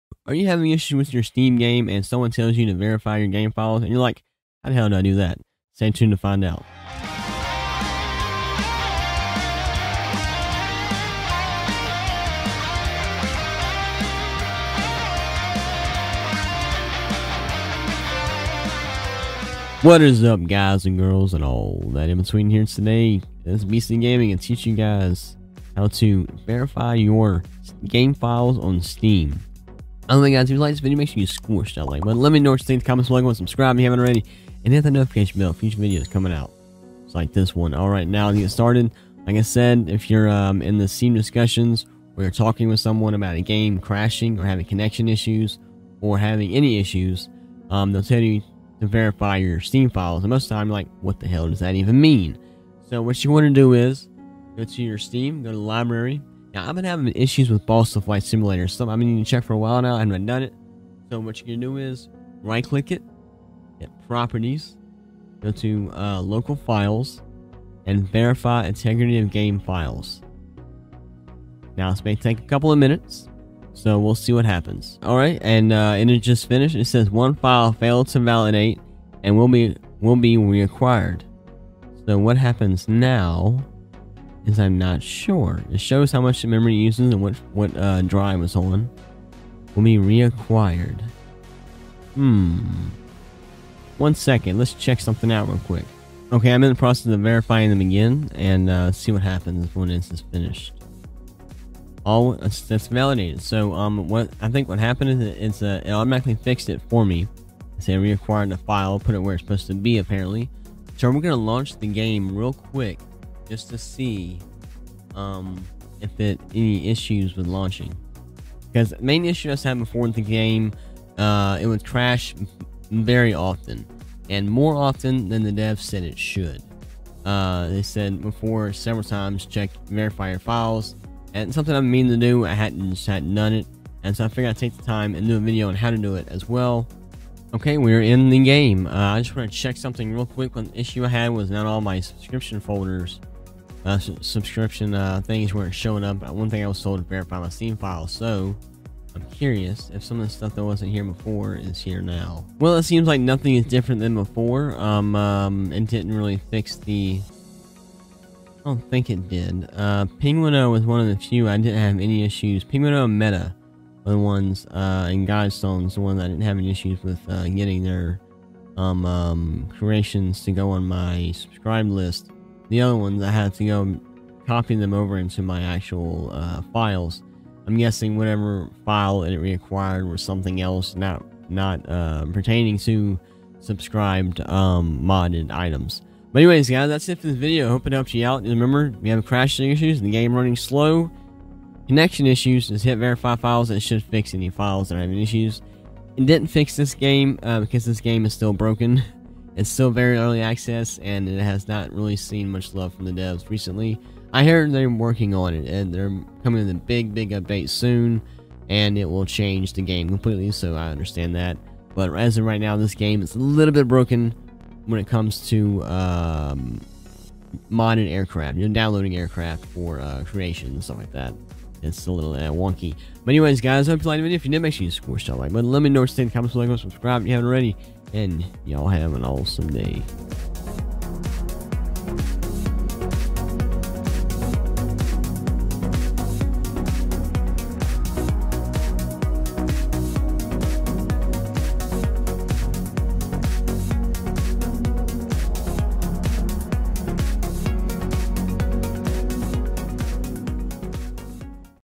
How To Verify Integrity Of Game Files On Steam.
LEARN HOW TO VERIFY FILES ON STEAM

By: Eveningland